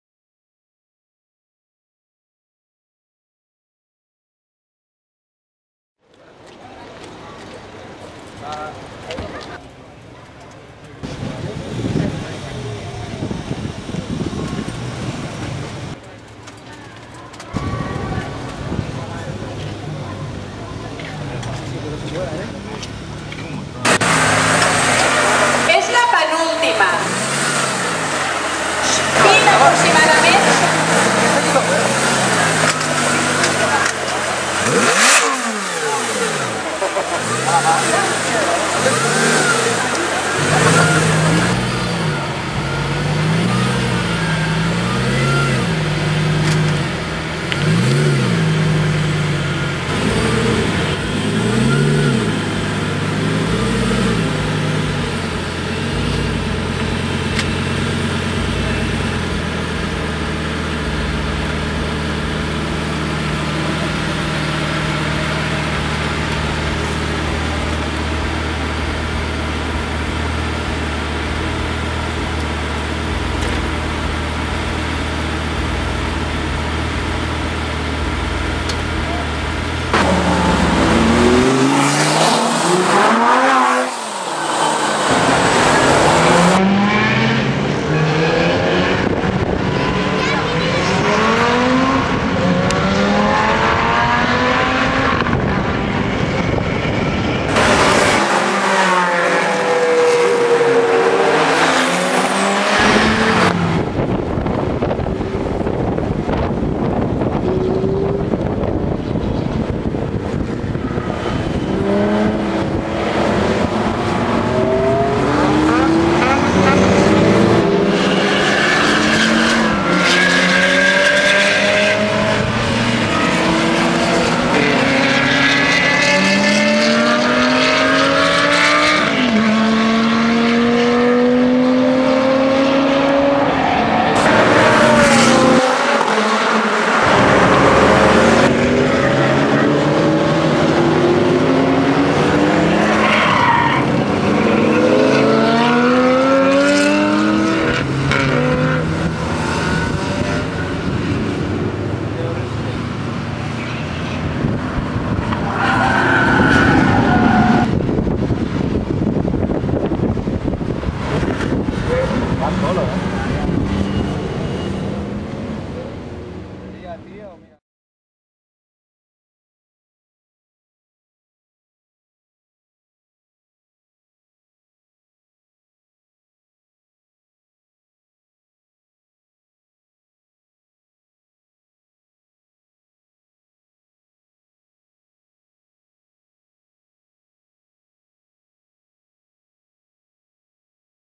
GREEN Lamborghini Gallardo Spyder Lovely REV and Driving on the Track!
GREEN Lamborghini Gallardo Spyder insane REV and drive on truck amazing and awesome exhaust sound engine full hd quality 1080p